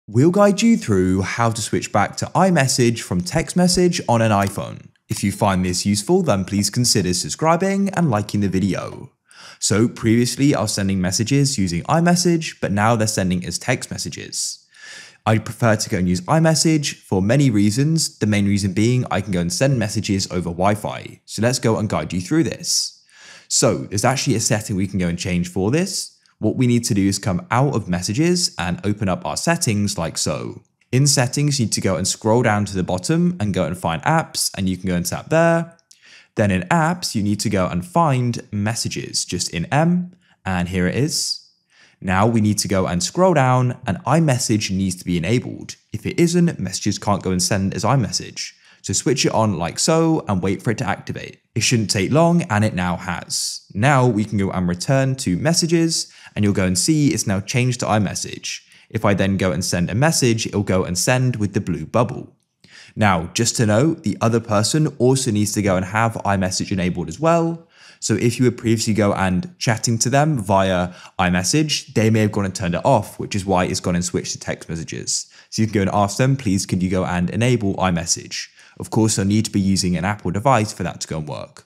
How To Switch To iMessage From Text Message On iPhone - Full Guide
Learn how to switch to imessage from text message in this video.

GuideRealm